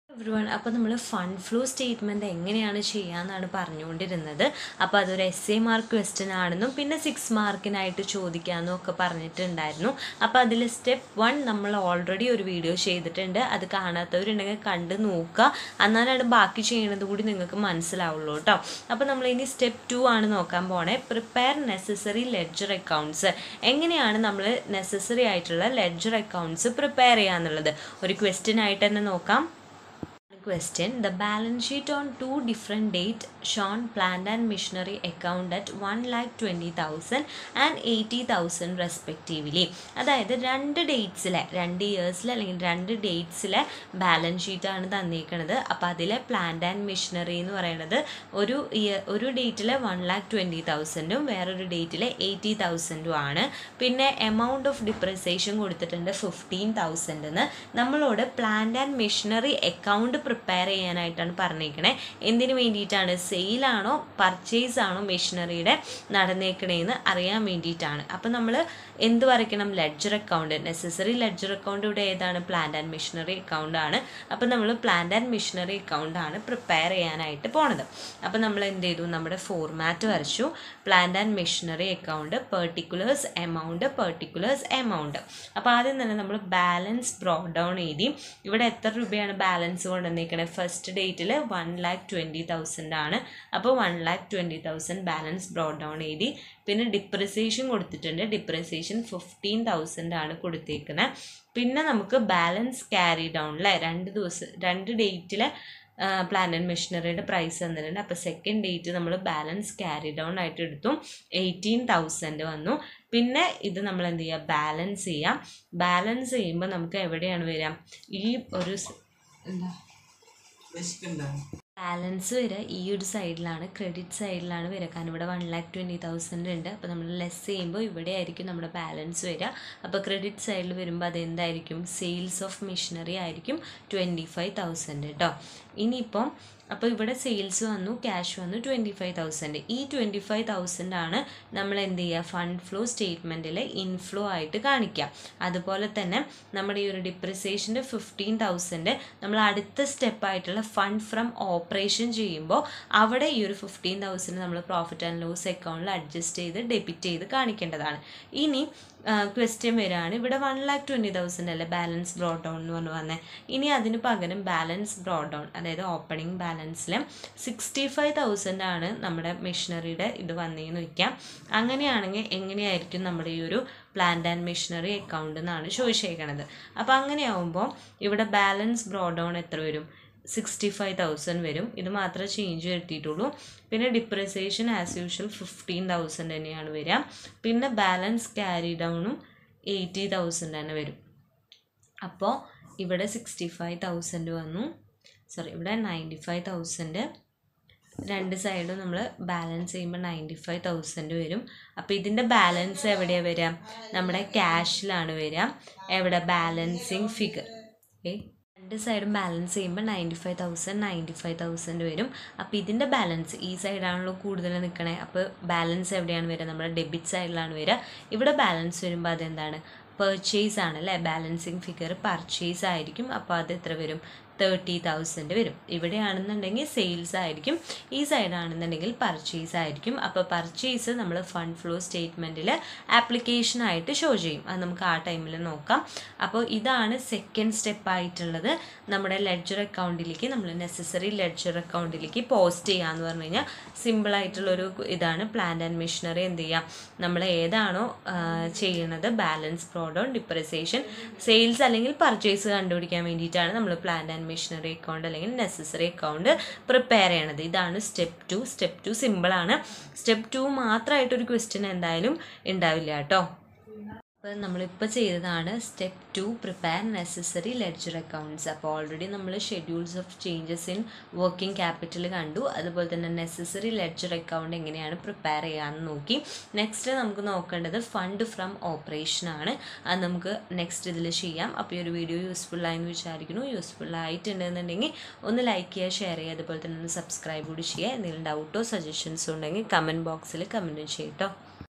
Calicut university, 5th sem, Accounting for management, fund flow statement, step 2,exam oriented
Calicut university
5th sem
accounting for management
fund flow